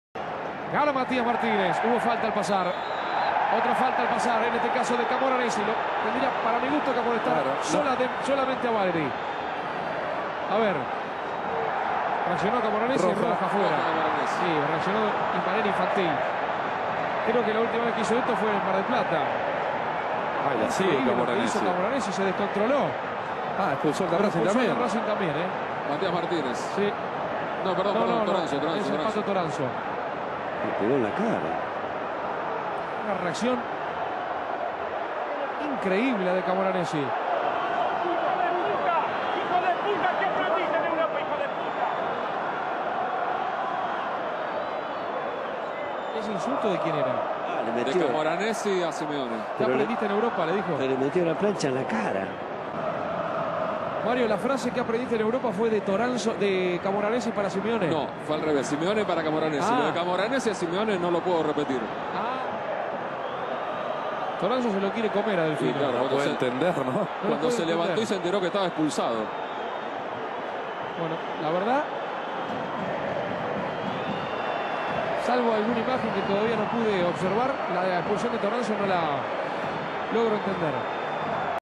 La Patada de Camoranesi a Toranzo ( Racing vs Lanus ) en HD
Patada de Camoranesi ,en la Cara, a Toranzo ( Racing vs Lanus ) en HD